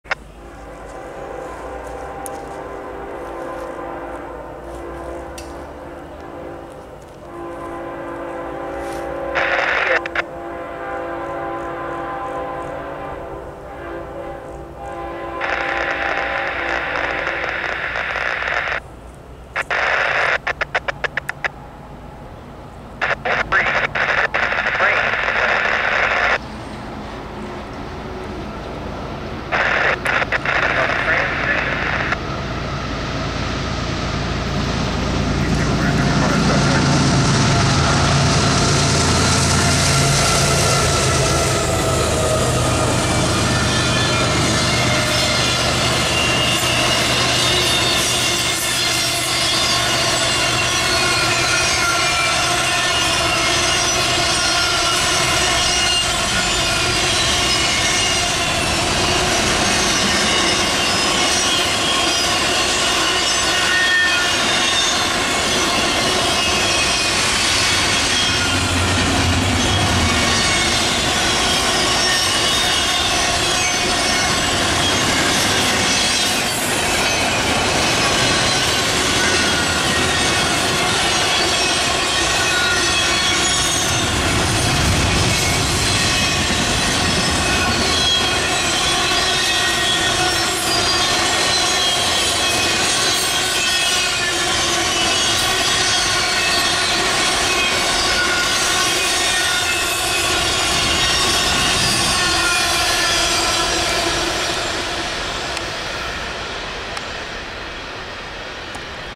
CSXT #3243 Leads Q032-06, Folkston, GA
-14:11: Q032-06 (Jacksonville, FL – North Bergen, NJ)
Northbound Intermodal
CSXT #3243 (ES44AH)
CSXT #866 (ES44AH)
228 axles, 5,300 feet